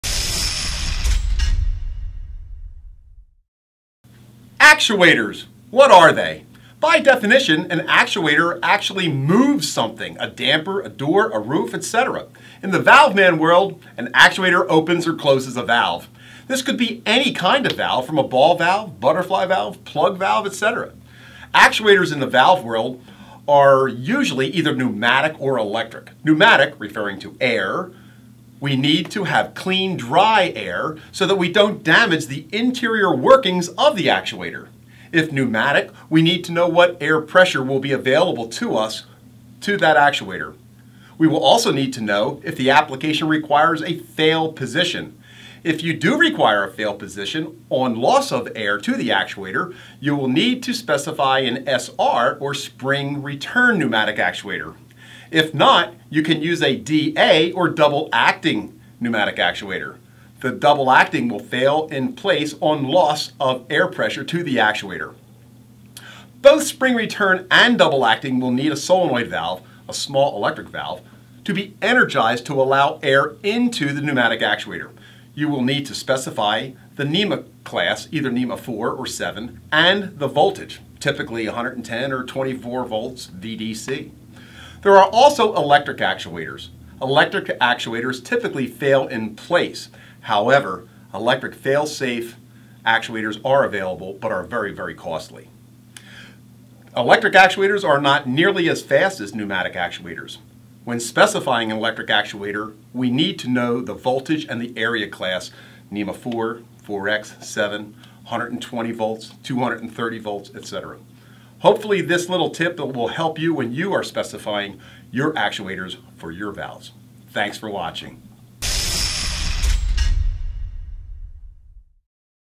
Valveman: What is an actuator?
The Valve man explains what an actuator is and and discusses the different types.